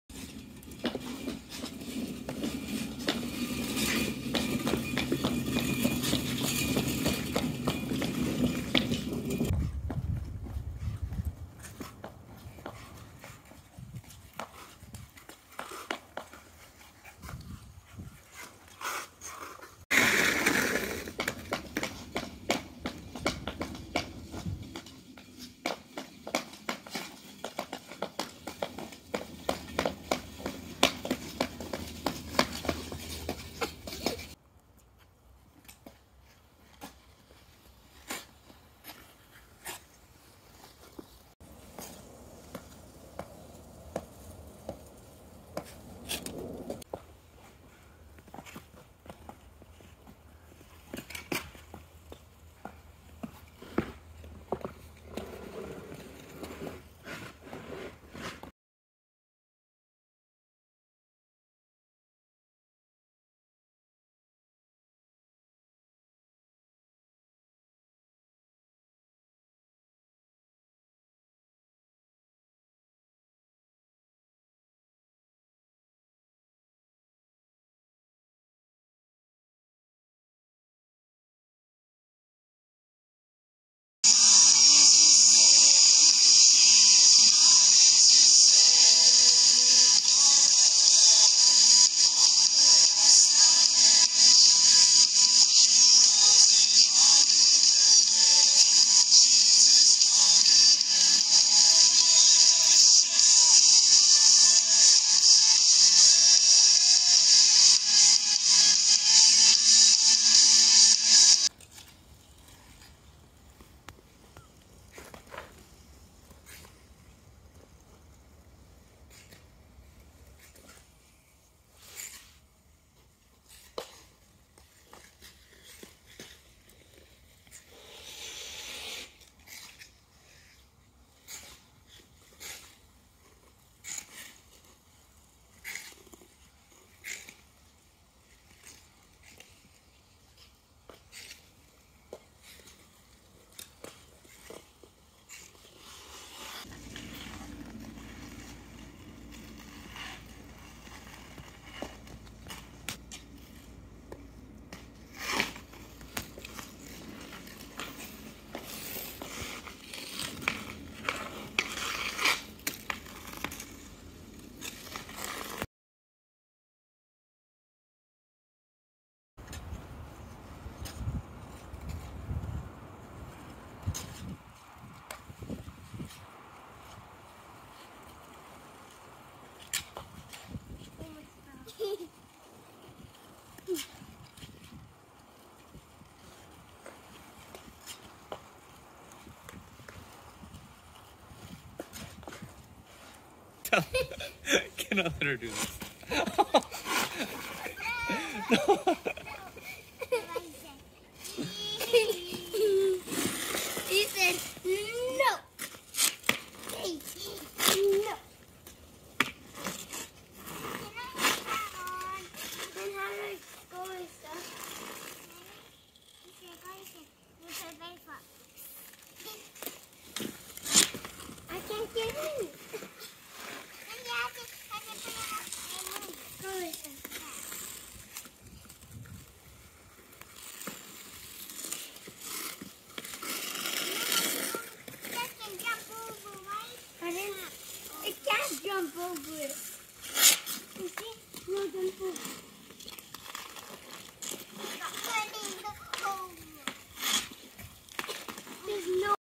NO&IS - Noah and Isabella learning how to ice skate!
This video chronicles Noah and Isa learning to skate on their own over the spam of one winter on our DIY backyard ice rink. They fell a few times, but they had so much fun!!!